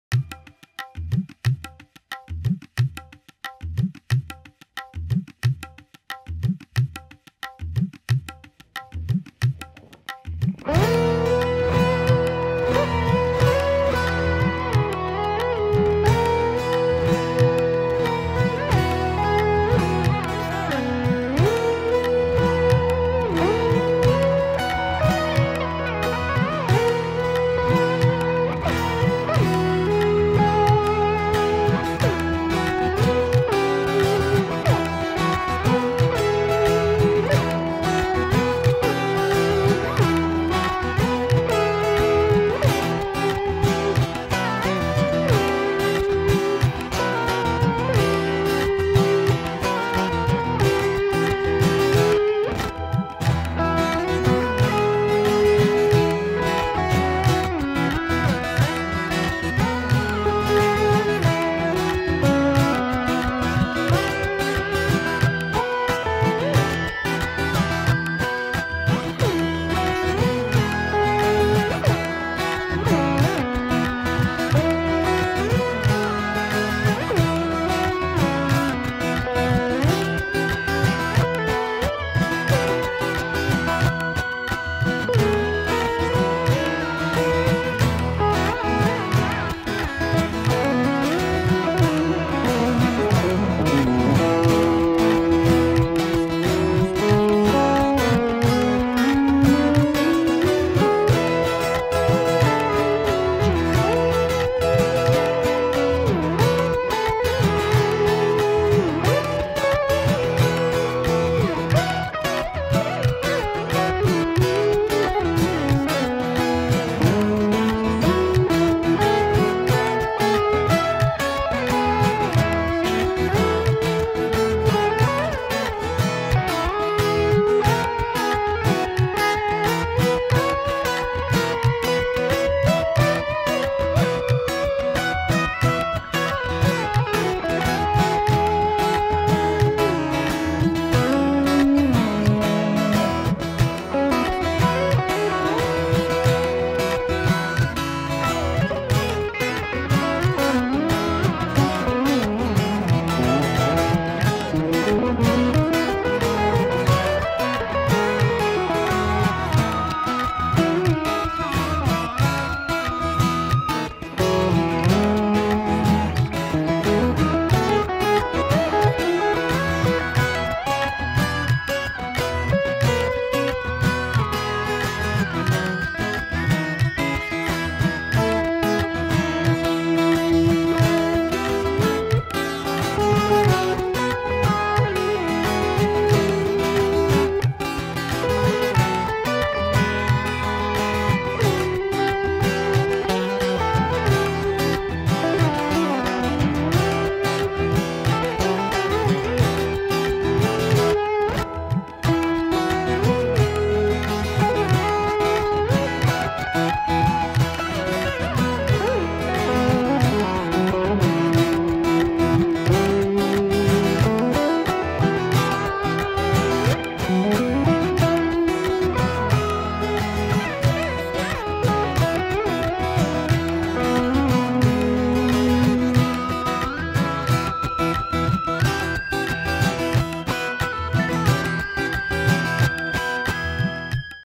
Carnatic guitar
This is a song in open tuning I wrote in the nineties ; I just discovered that its tuning is used in carnatic music, so I had to re-record it ! Rhythm guitar recorded with the piezo pickup of a PRS hollowbody, tuned (from high to low) in E-B-E-B-E-E. Solo improvisations with the Sustainiac pickup of my new Schecter Sun Valley Super Shredder FRSG, tuned in Eb.